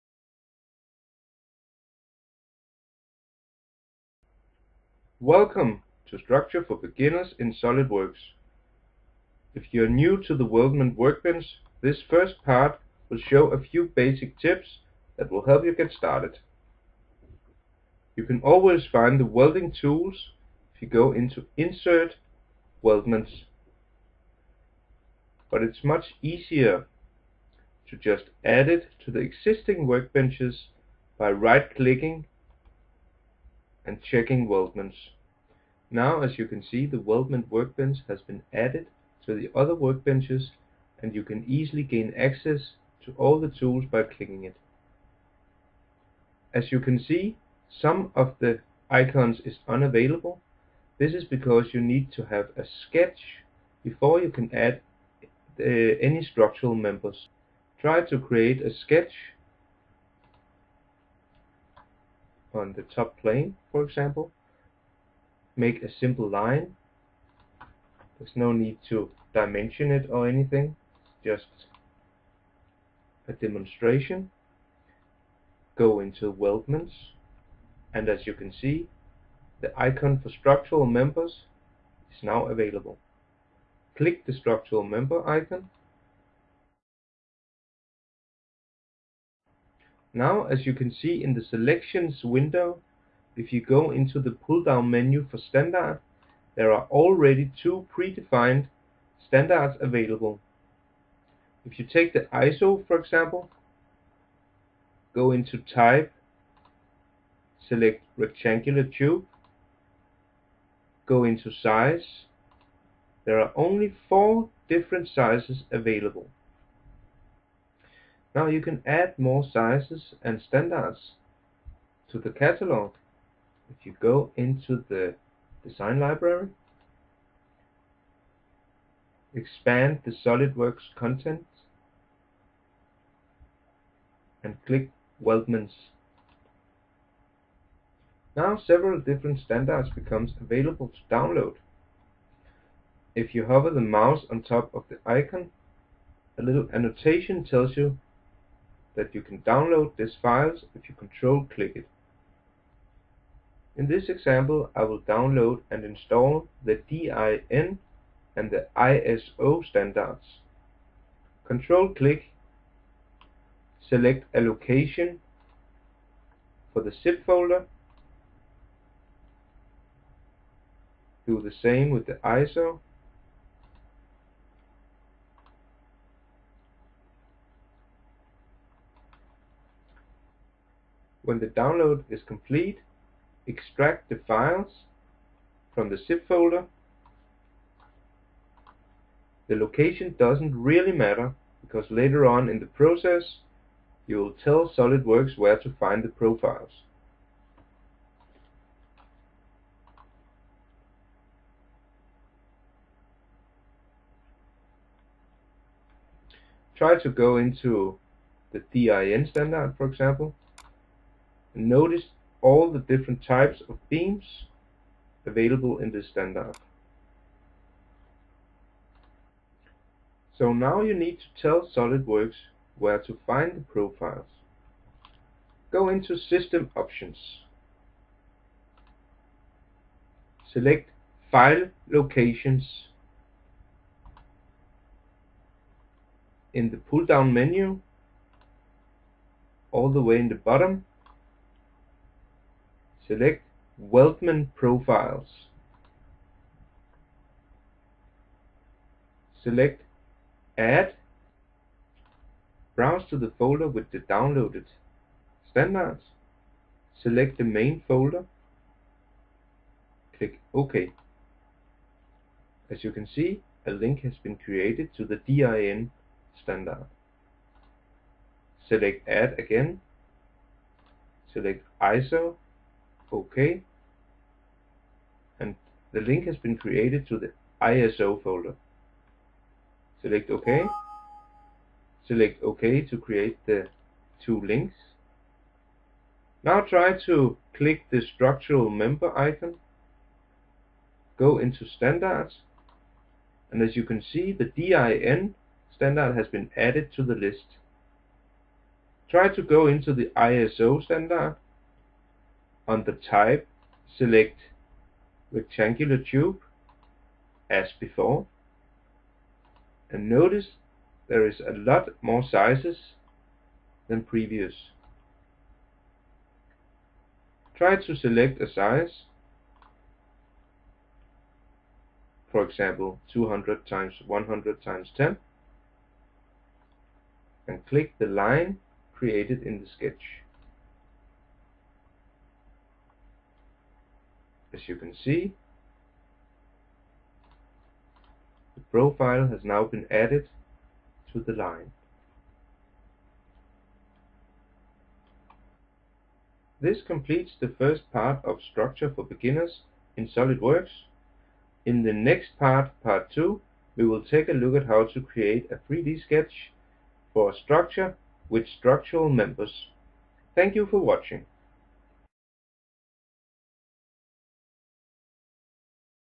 SolidWorks Tutorial: Structure For Beginners. Part 1.
This video shows how to download & apply DIN & ISO standard beams to the weldment library, so the list of beams variations becomes larger than the standard setup in SolidWorks.